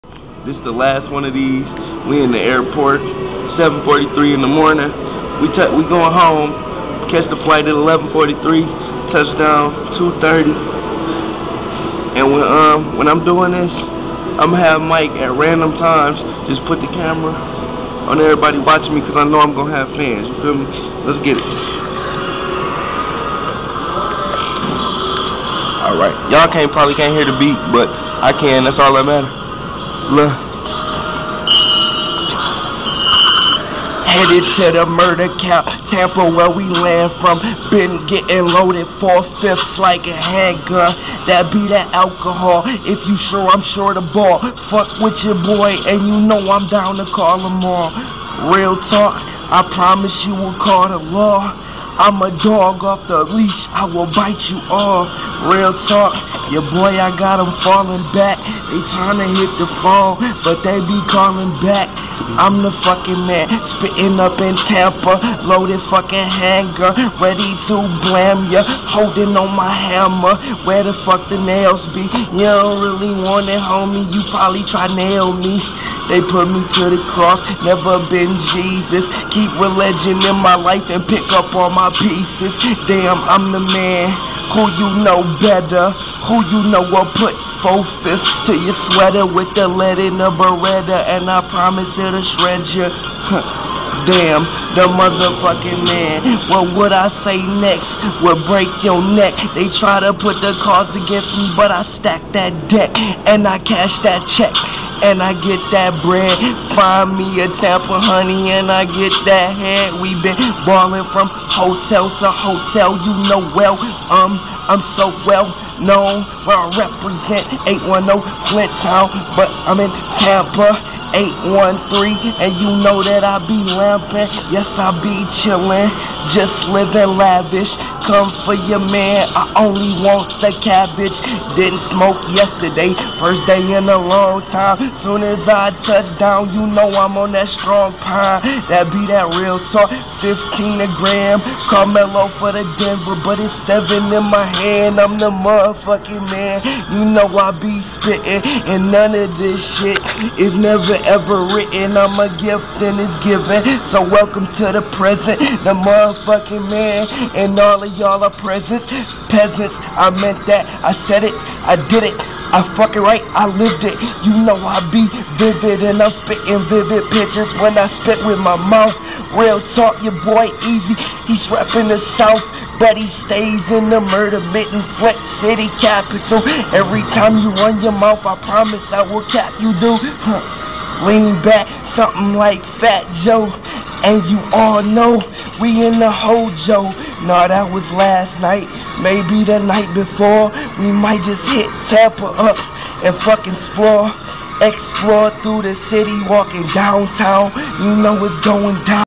EaSY Freestyle at the Tampa International Airport
Watch the 2 dudes from da airport feelin it